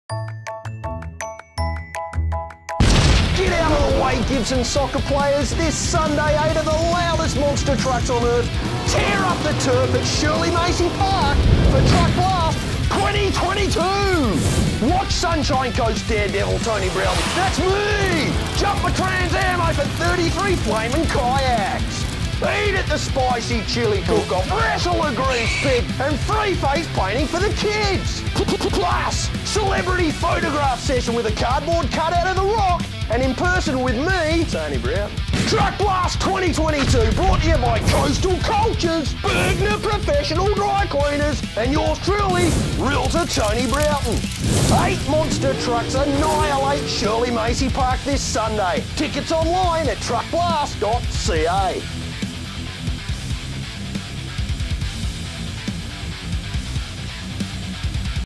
MONSTER TRUCKS at Shirley Macey Park in Gibsons, BC! - April Fools' Prank 2022
After creating an annual April Fools' Day video for 5 years, I recognized a pattern had developed: for reasons I can't explain, I find joy in antagonizing the very kind, very polite, very nice residents of the Sunshine Coast, BC.

That's why, in 2022, I threated to destroy a Gibsons park that's primary used for kids' soccer by hosting a monster truck rally.

In my defense, monsters trucks are really cool.

Realtor Tony Browton - Personal Real Estate Corporation⁠
RE/MAX City Realty (Gibsons)⁠

VIDEO BY:
Burnt Boat Media - Gibsons, BC
Real estate photos and videos on the Sunshine Coast, BC